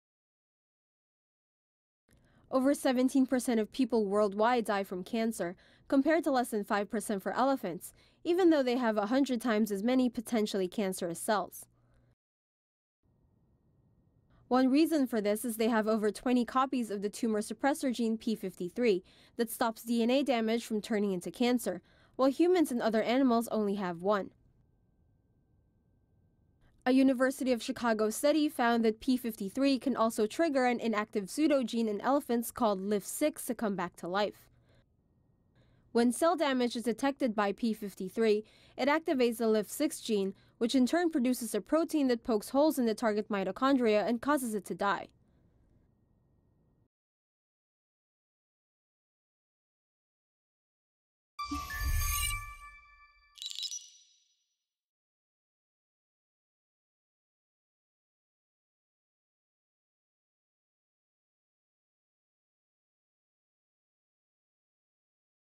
'Zombie gene' protects elephants from cancer
Elephants rarely get cancer, and scientists may have finally figured out why.

According to research from the University of Chicago, over 17 percent of people worldwide die from cancer, compared to less than 5 percent for elephants, even though they have 100 times as many potentially cancerous cells.

One reason for this is they have over 20 copies of the tumor suppressor gene p53 that stops DNA damage from turning into cancer, while humans and other animals only have one.

Because the cells died as soon as damage was detected, there is zero risk of them ever becoming cancerous.

The study found that p53 can also trigger an inactive pseudogene in elephants called LIF6 to come back to life. When cell damage is detected by p53, it activates the LIF6 gene, which in turn produces a protein that pokes holes in the target mitochondria and causes it to die.

This genetic resistance to cancer is not just seen in elephants. Smaller critters like naked mole rats and grey squirrels also have mechanisms that keep them tumor-free.

By studying how these animals' genes work, scientists are hoping to figure out how to prevent or treat cancer in humans.

RUNDOWN SHOWS:
1. Cancer mortality rates in humans and elephants
2. Elephants having more copies of tumor suppressor gene p53
3. p53 reanimating inactive LIF6 pseudogene
4. LIF6 gene triggering cell death

VOICEOVER (in English):
"Over 17 percent of people worldwide die from cancer, compared to less than 5 percent for elephants, even though they have 100 times as many potentially cancerous cells."

"A University of Chicago study found that p53 can also trigger an inactive pseudogene in elephants called LIF6 to come back to life."

"When cell damage is detected by p53, it activates the LIF6 gene, which in turn produces a protein that pokes holes in the target mitochondria and causes it to die."